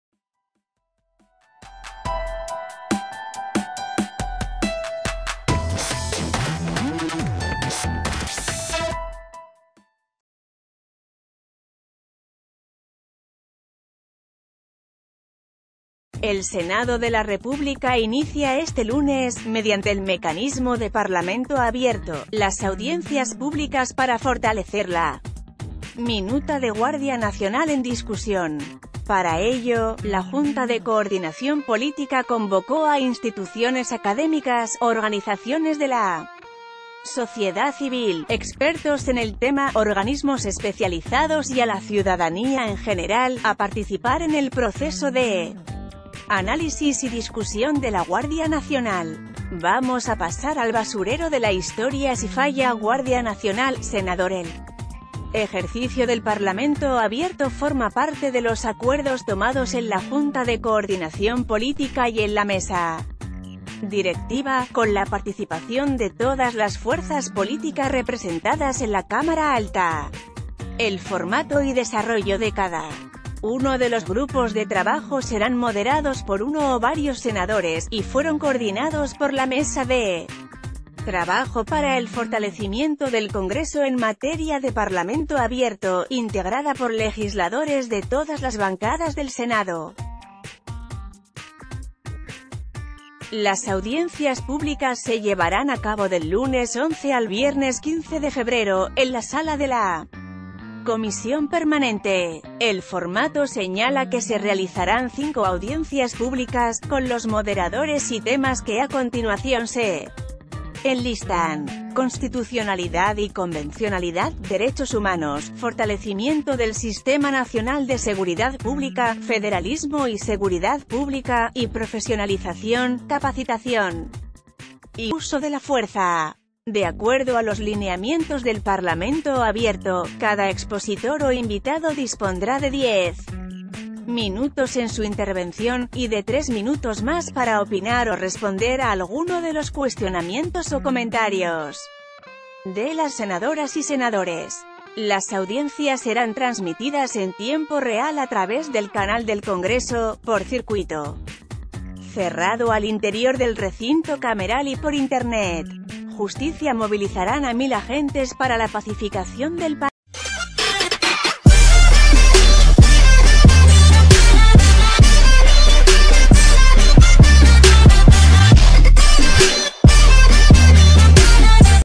Este lunes inician audiencias públicas en el Senado para discutir la Guardia Nacional
El Senado de la República inicia este lunes, mediante el mecanismo de Parlamento Abierto, las audiencias públicas para fortalecer la Minuta de Guardia Nacional en discusión. Para ello, la Junta de Coordinación Política convocó a instituciones académicas, organizaciones de la sociedad civil, expertos...